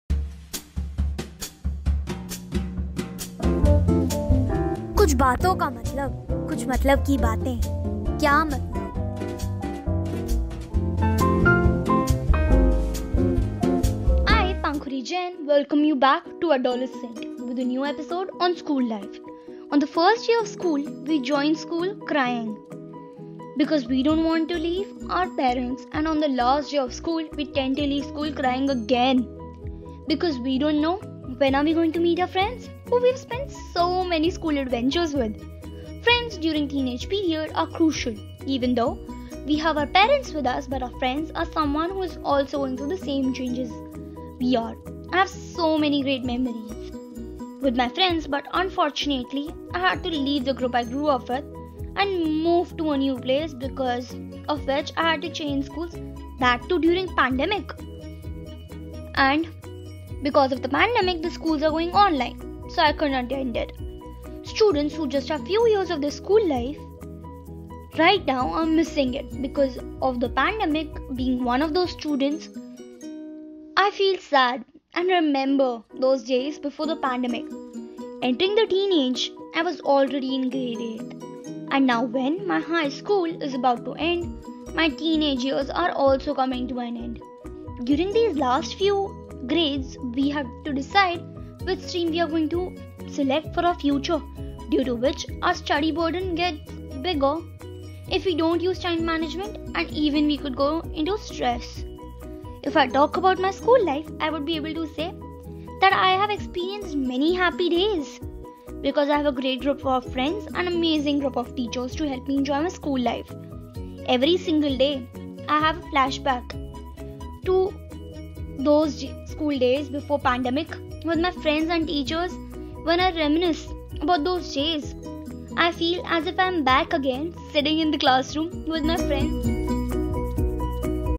SCHOOL LIFE
SHOW :ADOLESCENT

HOST: Pankhuri Jain
Section: JADI BUTI
Episode:SCHOOL LIFE

Pankhuri Jain is a student at - GD GOENKA SIGNATURE SCHOOL
and is super talented.Her areas of interest are Interests- taekwondo, dance and listening music.
Her show Adolescent is about different emotions & challenges faced by teenage kids.